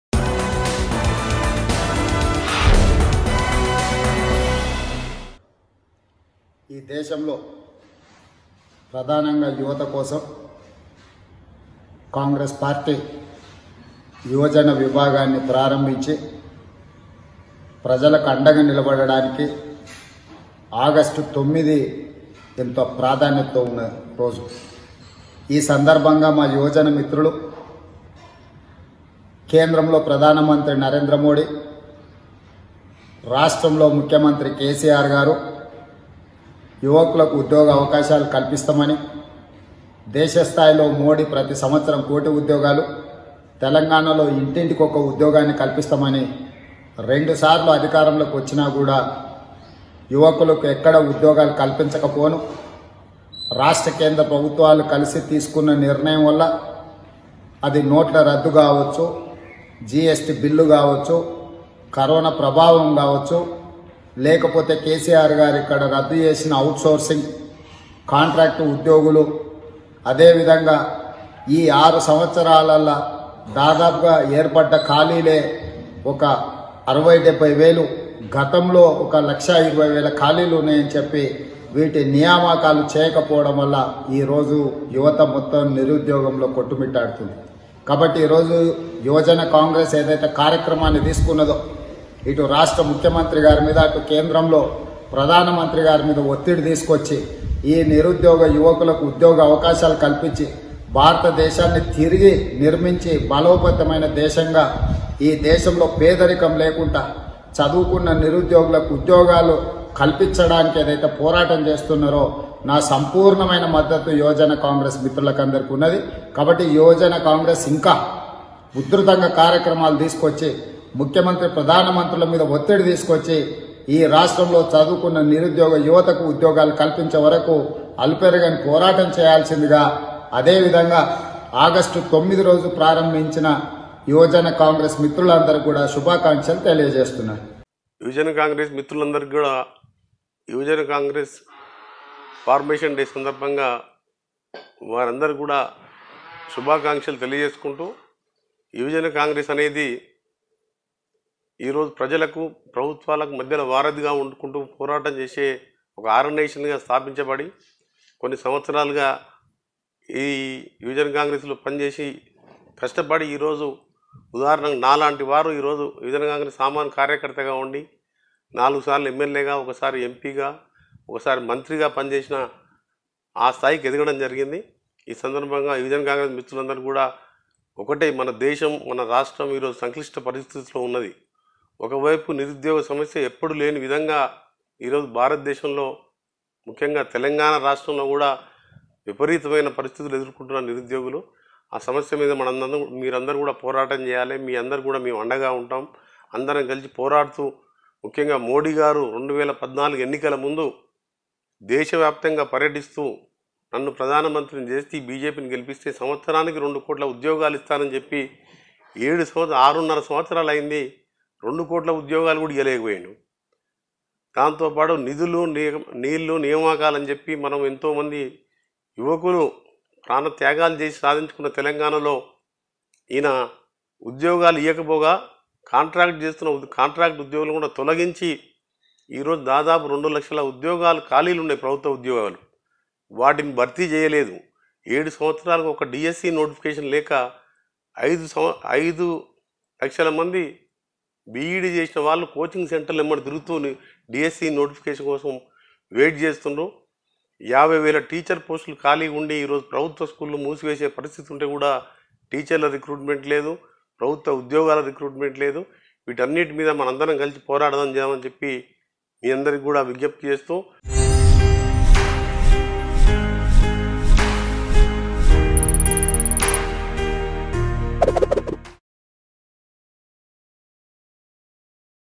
MP Revanth Reddy Says To Fight Against PM Modi & CM KCR Ruling | Telangana Politics | Mango News
MP Revanth Reddy Says To Fight Against PM Modi & CM KCR Ruling.